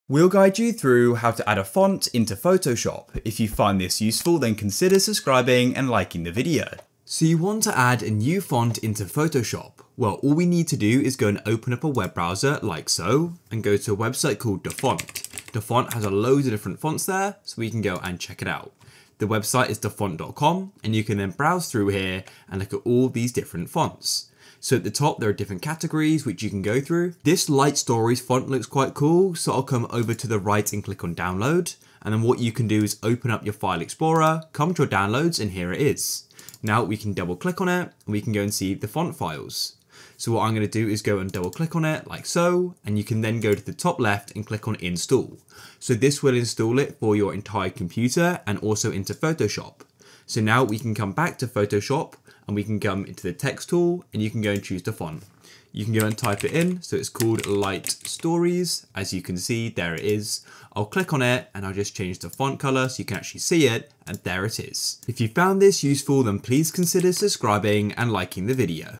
How To Add Fonts In Photoshop - Full Guide
Learn how to add and install new fonts in photoshop in this video .

GuideRealm